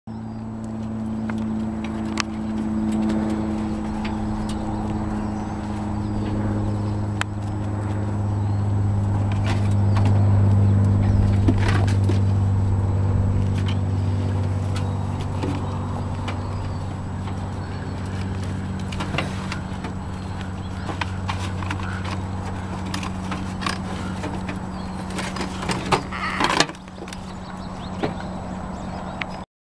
Gertrude's Kids 008
The golf cart is for Keith's father to drive around the farm. He puts feed on the deck where golf bags would go and the goats like to climb on the cart and eat the left-overs.